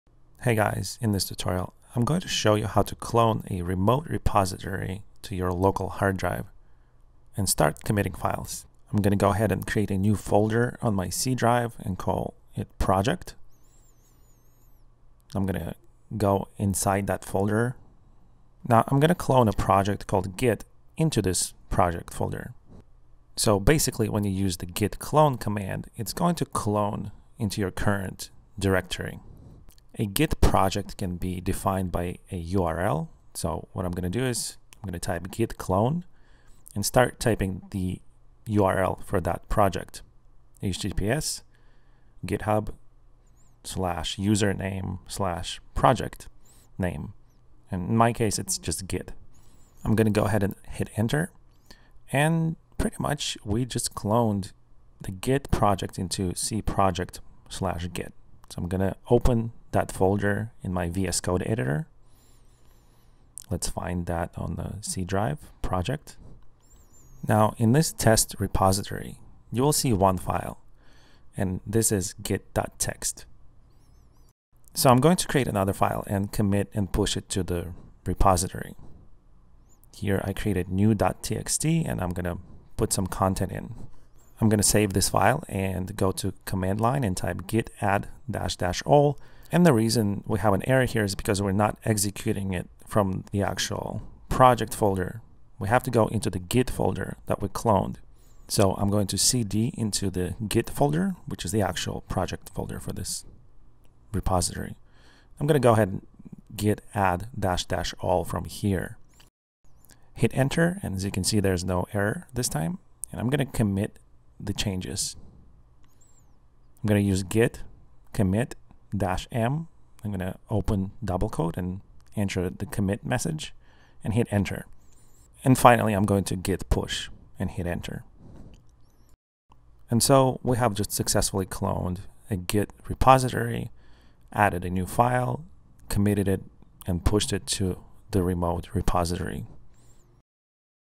How To Clone Git Project (The right way)
git clone remote repository is done when you wish to grab a copy of a github project and copy it to your hard drive for committing and pushing new changes back to the remote repo. When you clone a repo you don't need to git init a new project, like you would, if you were starting the repo locally.

00:00 git clone remote repository
00:10 navigate to your project on c drive (or elsewhere)
00:32 git clone
0:56 opening project folder in vscode
1:16 modifying project text file
1:21 git add all
1:52 git commit -m "add commit message"